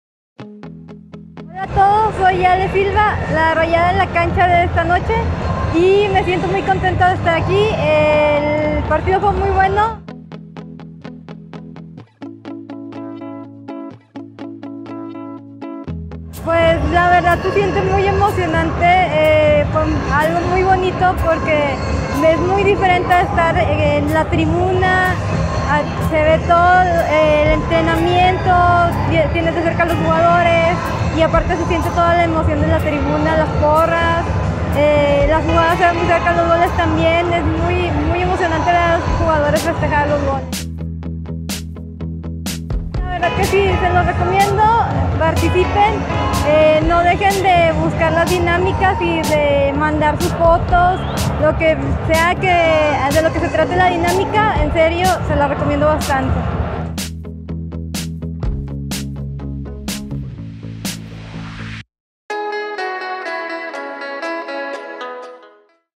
RayadoEnLaCancha Alejandra Silva J9 CL2020
Ale Silva fue la ganadora de la dinámica Rayado en la Cancha y estuvo presente en la Jornada 9.